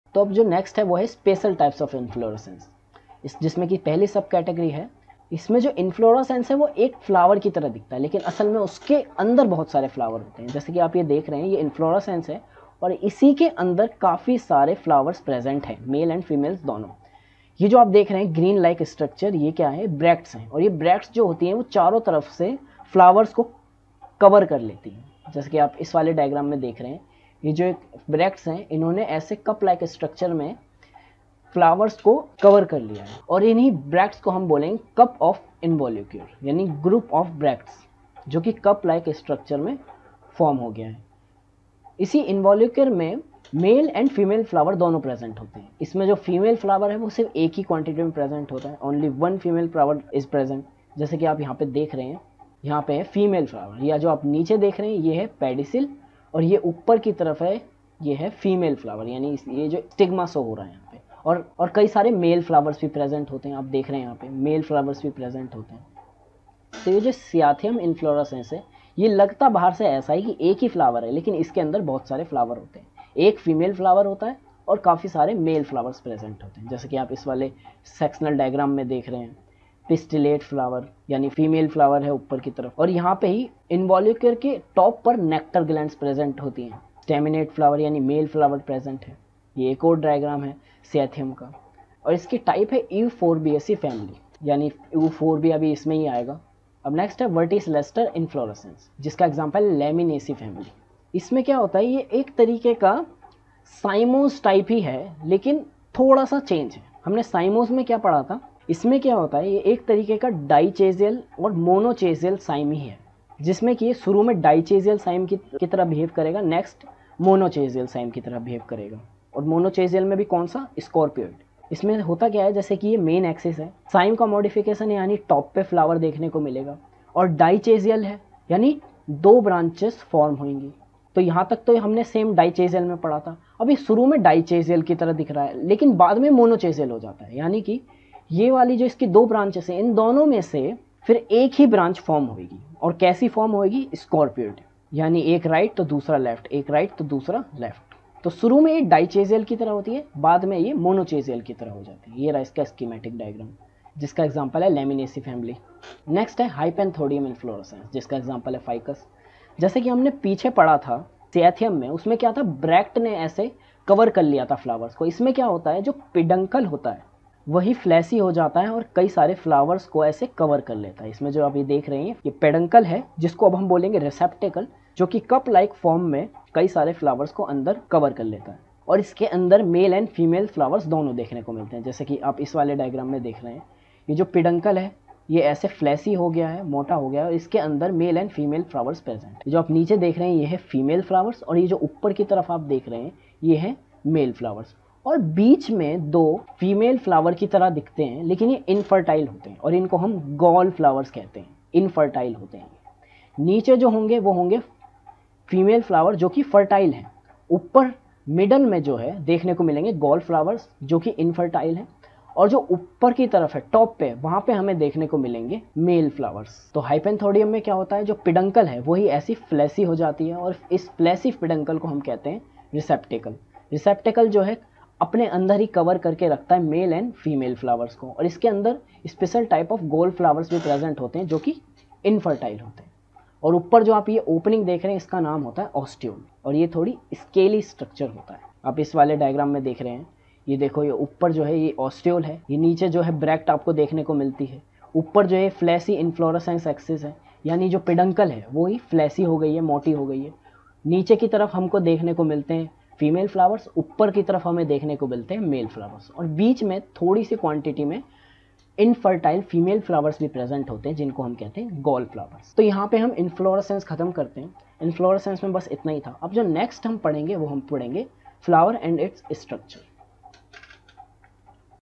09 SPECIAL TYPE OF INFLORESCENCE || MORPHOLOGY OF FLOWERING PLANTS || NEET
TOPICS COVERED
CYATHIUM
HYPANTHODIUM
VERTICILLASTER